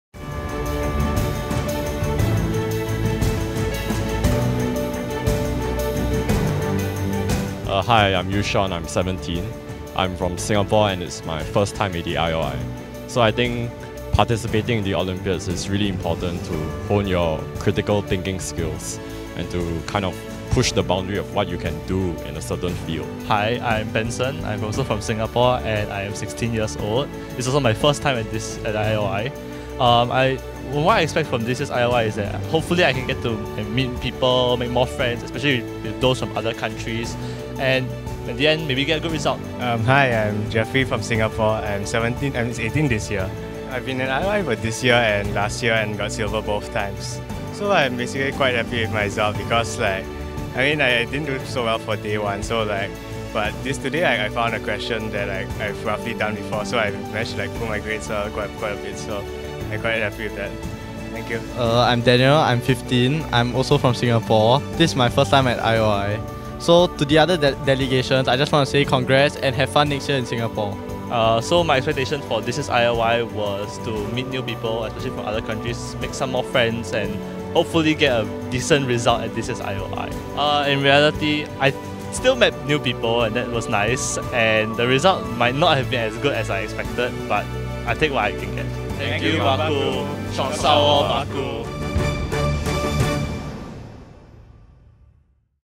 IOI 2019: Team interview - Singapore
Team Singapore at IOI 2019:
Benson Zhan Li Lin (Bronze medal)
Daniel Zhenghao Choo
Eu-Shaun Leong (Gold medal)
Jeffrey Chun Hean Lee (Silver medal)

Leader: Steven Halim
Deputy Leader: Li Ling Wang